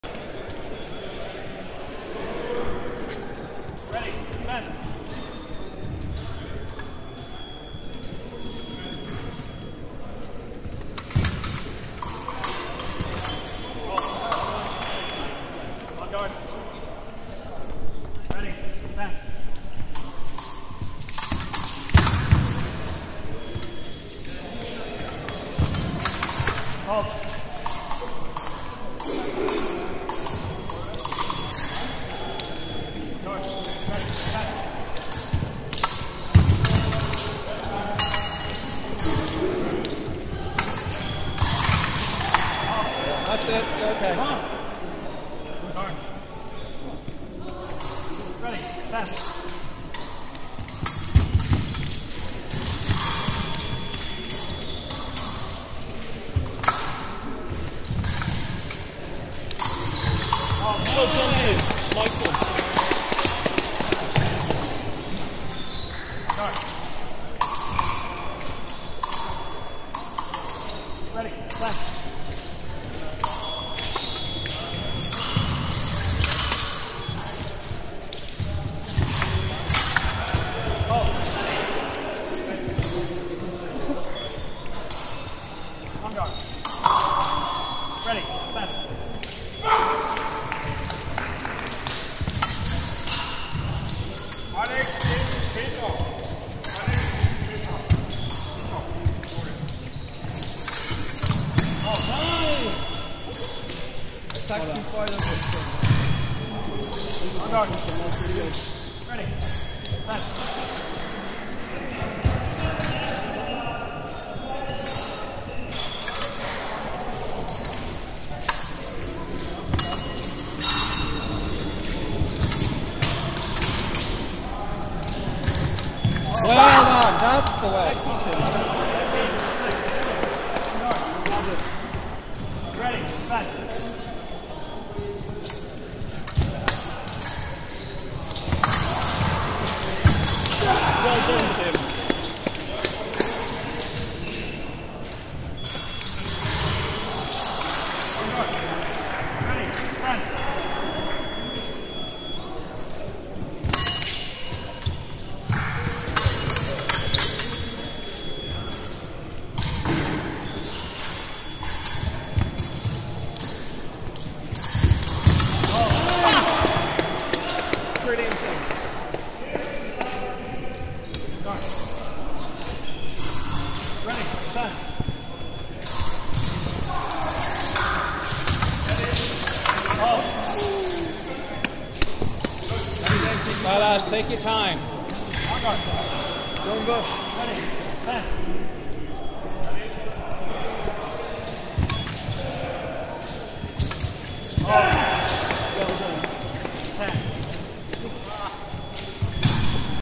Duffy's team competition 24 January 2015
To celebrate the 27th Professor Duffy Memorial Team Epee competition, Ireland's Premier International Epee event.

Venue: Trinity College, Dublin, Sports Hall